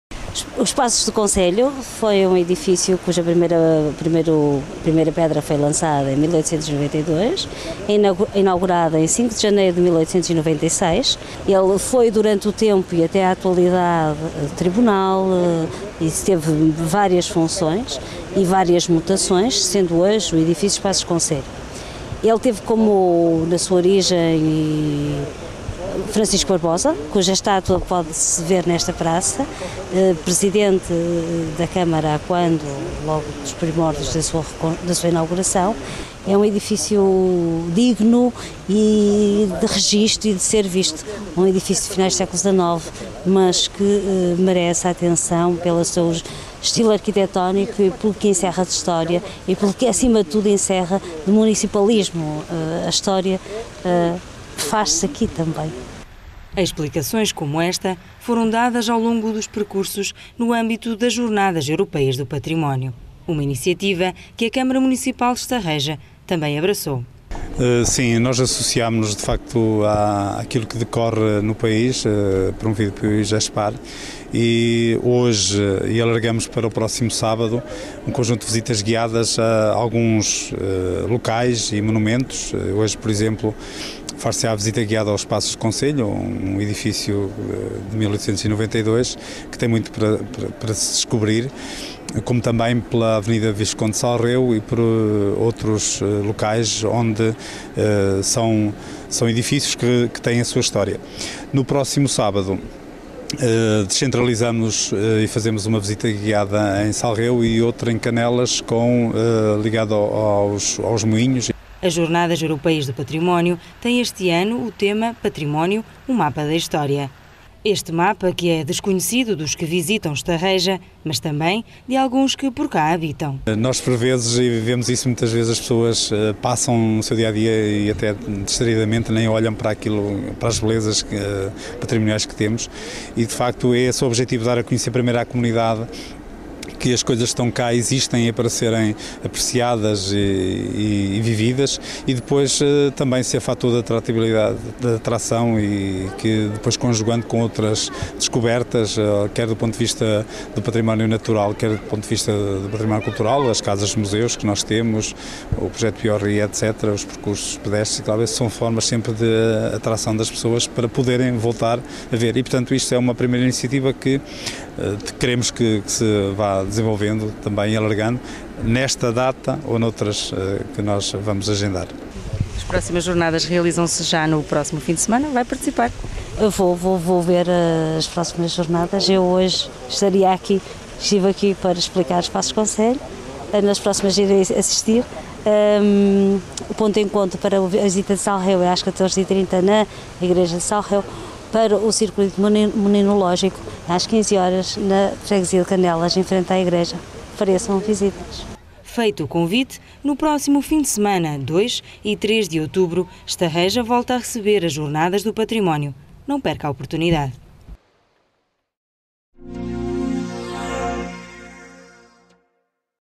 Património edificado de Estarreja
O Município de Estarreja associou-se às Jornadas Europeias do Património, em setembro de 2010, com visitas guiadas e gratuitas ao património histórico local, designadamente ao centro da cidade de Estarreja, com passagens pela Casa dos Morgados de Santo António da Praça, Paços do Concelho de Estarreja, Praça Francisco Barbosa e Avenida Visconde de Salreu e aos Moinhos de Canelas e ao Centro Cívico de Salreu.